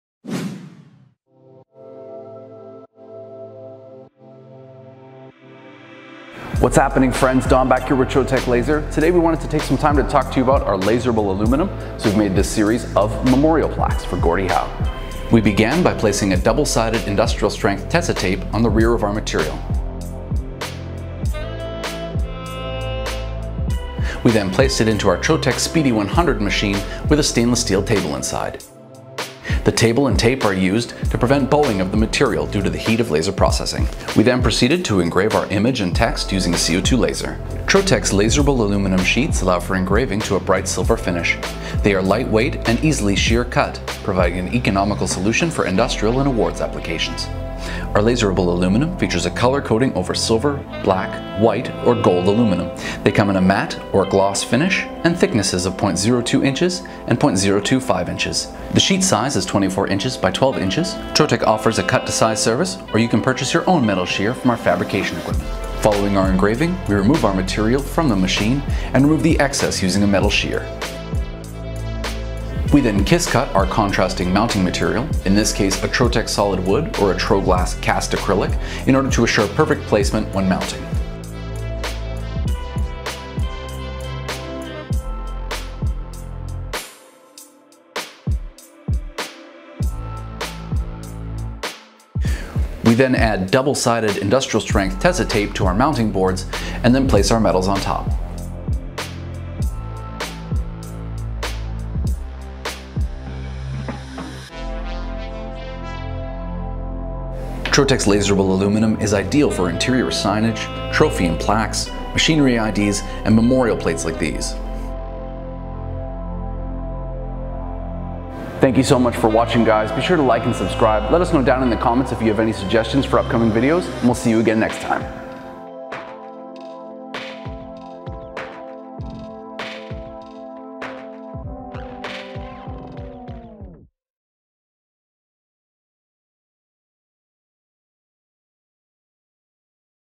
Laser Engraving Memorial Plaque | Laserable Aluminum | Trotec
This week, we're laser engraving a photograph and text memorial plaque on our Trotec laserable aluminum products. We'll show you how our CO2 laser can engrave on aluminum.

We first take our industrial double-sided Tessa tape to place the laserable aluminum onto our stainless steel table. After engraving it, we then take out some excess material with our metal cutting sheer, and then place the memorial plaque onto wood and acrylic.

Laserable aluminum sheets allow for laser marking and engraving to a bright silver finish. It is very lightweight and easy to cut, providing an economical solution for industrial applications and the awards industry.

***LASER SETTINGS***
Laser: Trotec Speedy 100 80w
Engraving: 100 power, 70 speed